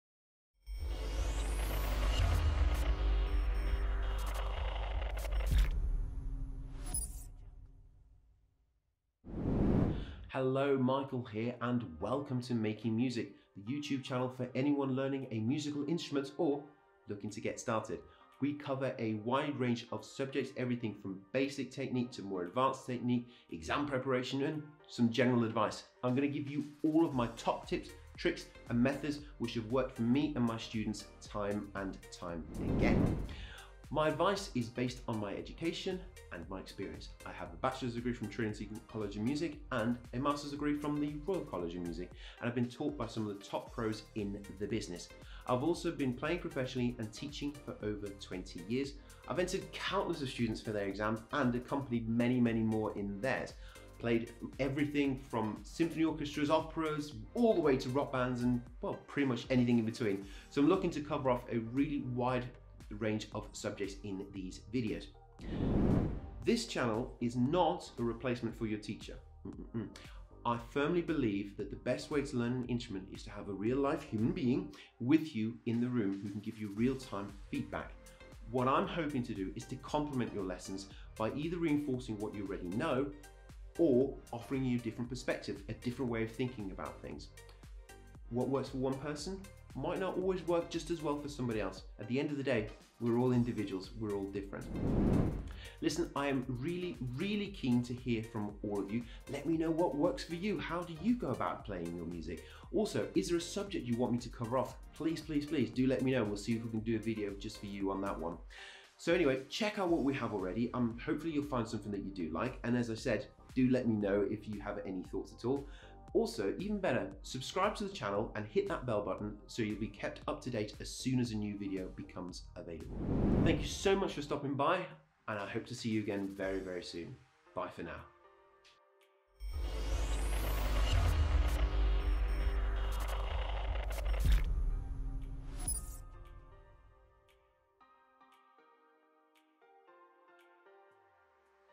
Learning Musical Instruments? Making Music can help you | Introduction
Hello, amazing viewers,
This video is all about what we do and what is Making music:
We will cover a wide range of subjects from basic techniques to advance, exam preparation and some general advice. Top tips tricks and methods for learning musical instruments.